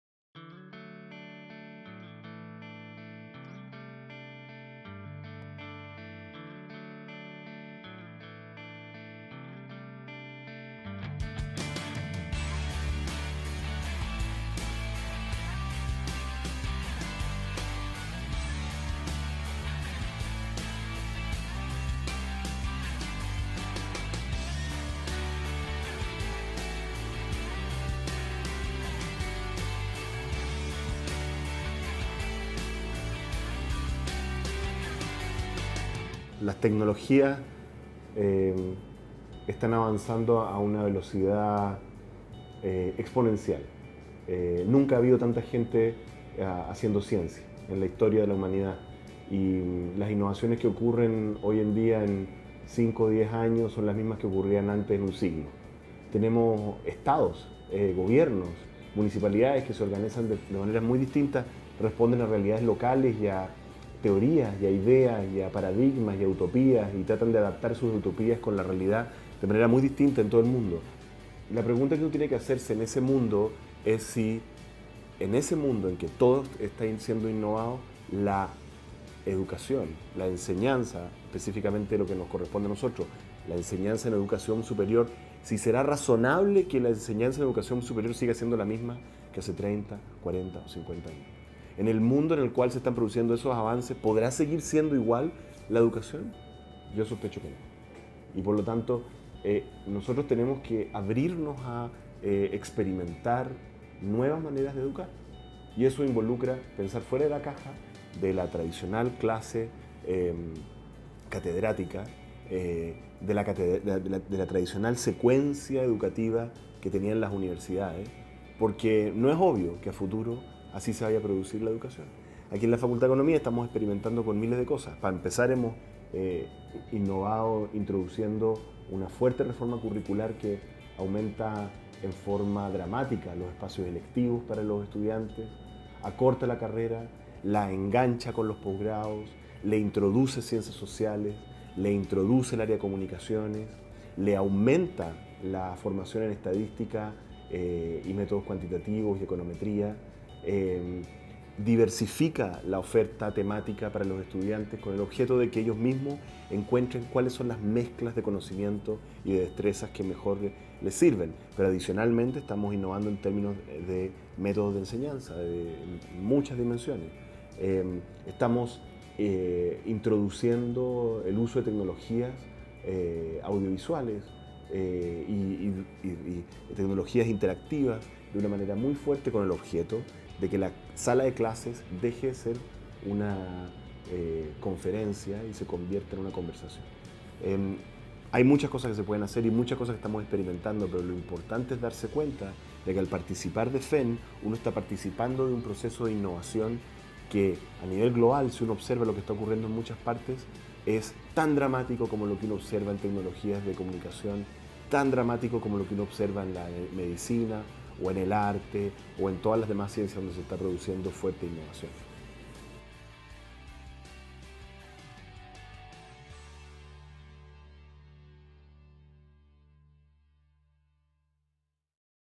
Docentes Innovadores FEN: Profesor Óscar Landerretche, Departamento de Economía
Video que relata la visión del Director de la Escuela de Economía y Administración, uno de los más destacados docentes de la Facultad de Economía y Negocios,  respecto a las razones por la cual la formación en la Educación Superior debe siempre estar en constante diálogo frente a los vertiginosos cambios del mundo actual.